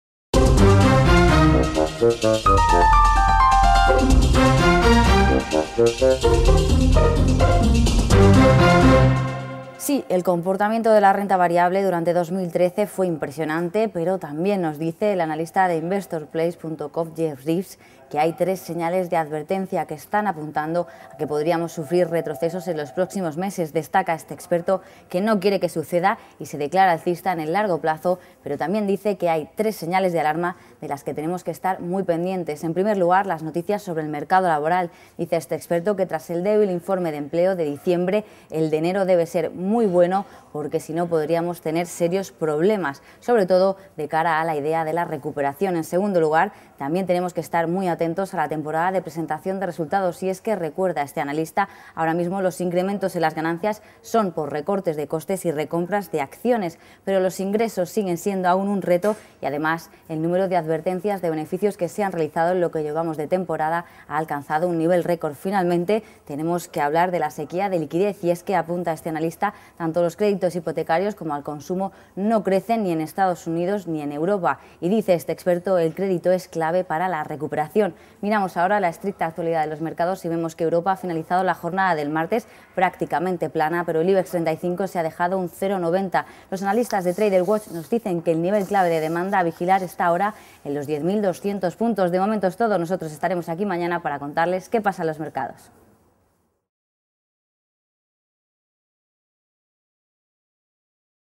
Radiografía de la sesión
"Tras la impresionante 'carrera' protagonizada por la renta variable durante 2013, hay algunas señales de alarma en el horizonte", advierte Jeff Reeves, editor de InvestorPlace.com. ¿Cuáles son? ¿Podríamos asistir a un pullback de cierto calado en los próximos meses?